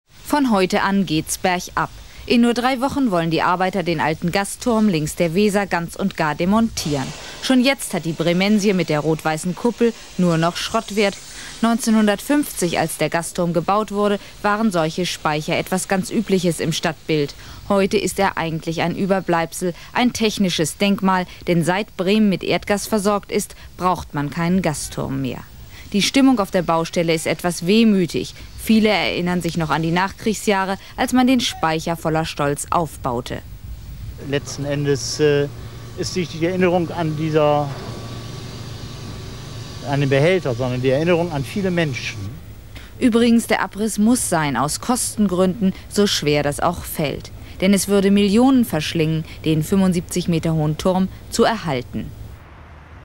Als 1984 der Gasturm Links der Weser mit Wehmut demontiert wurde
Mit Wehmut demontierten die Stadtwerke 1984 den damals 75 Meter hohen Gasspeicher-Turm aus der Nachkriegszeit.